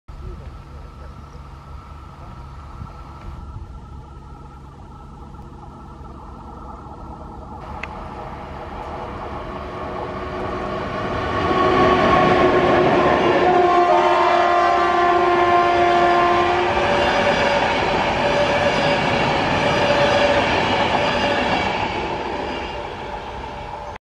EP09-009 + RP1
Epoka z pociągiem EIC 'Chrobry' zmierza w stronę Kutna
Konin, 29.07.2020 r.